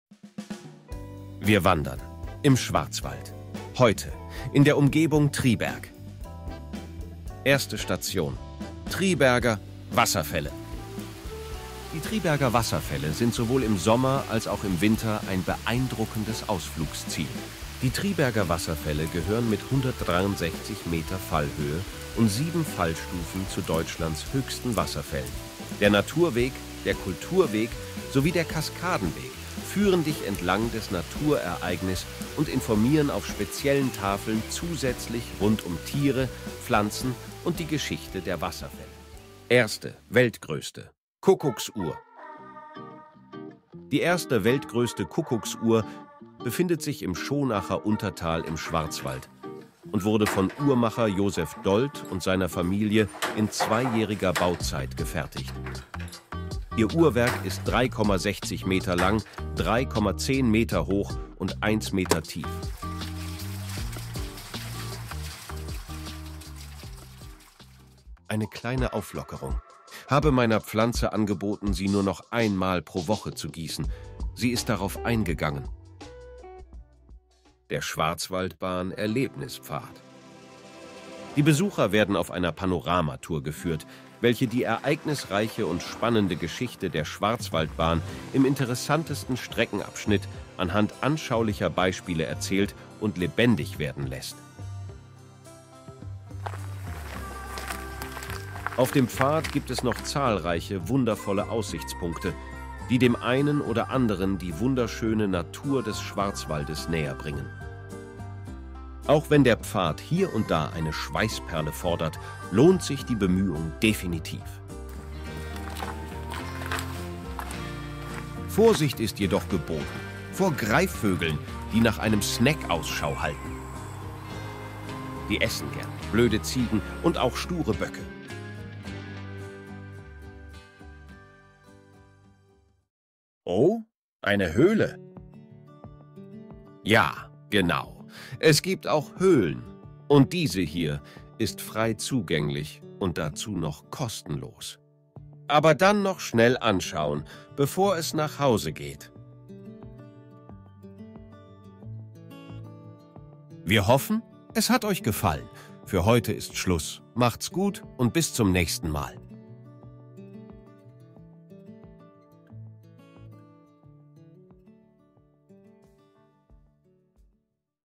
Dokumentation Wandern im Schwarzwald Triberg
Diese Dokumentation handelt über das Wandern im Schwarzwald in der Umgebung Triberg.

Inhalt der Wanderung:
1. Triberger Wasserfälle
2. Die erste Weltgrößte Kuckucksuhr
3. Schwarzwaldbahn-Erlebnispfad